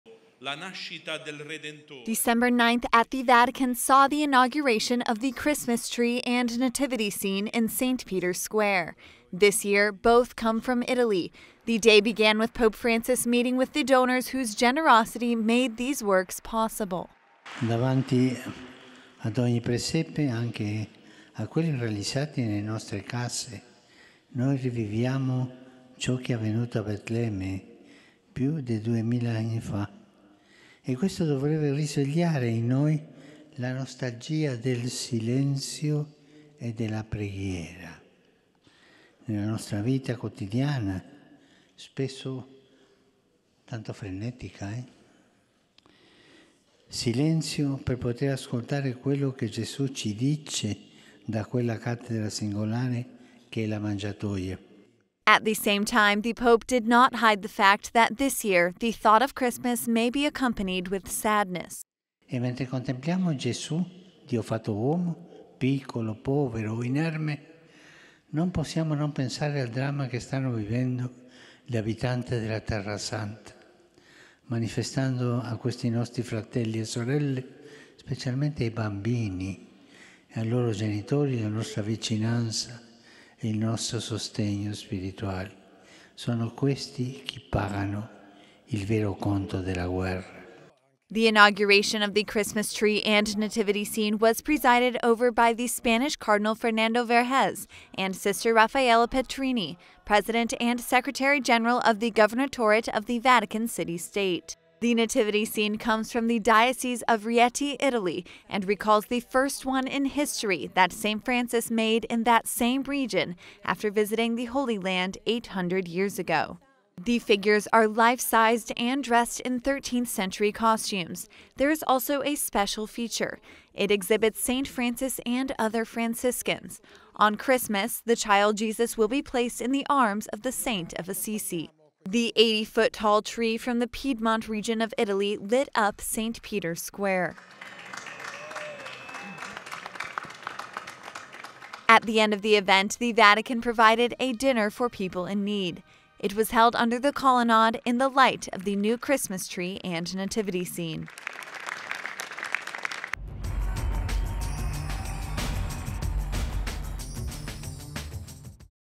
Vatican inaugurates nativity scene that reflects the first one in history
While addressing the donors of the Christmas tree and nativity scene, Pope Francis did not hide the fact that this year, the thought of Christmas may be accompanied with sadness.